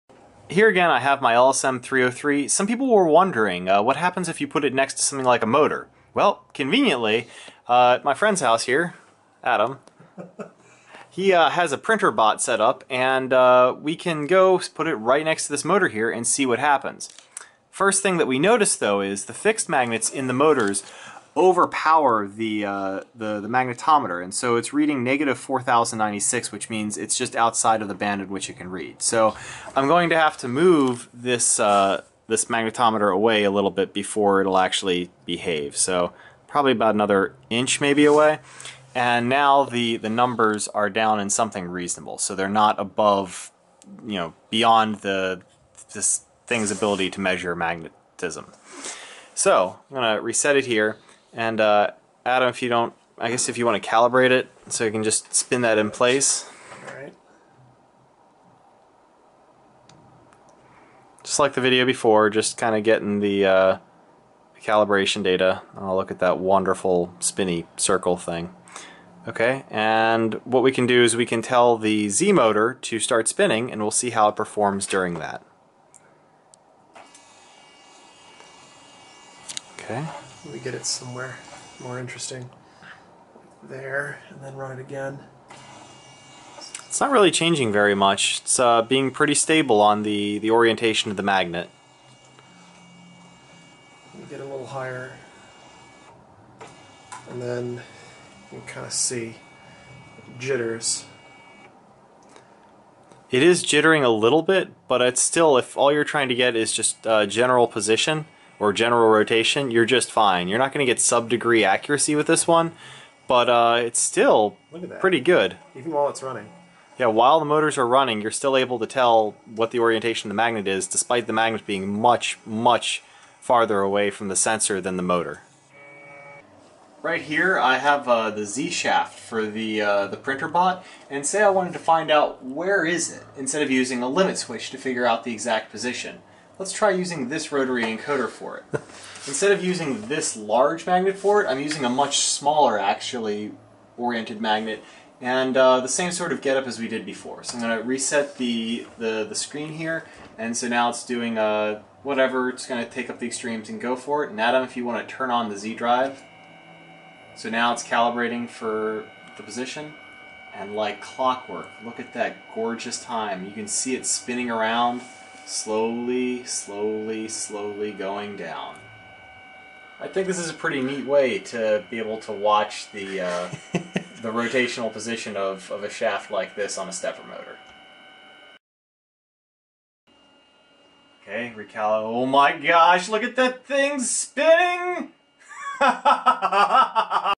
Magnetic rotary encoders with stepper motors
Some people wanted to know what happens when you get the magnetic rotary encoder near a motor.  Well I found myself near one and decided to find out.  Answer: It works surprisingly well.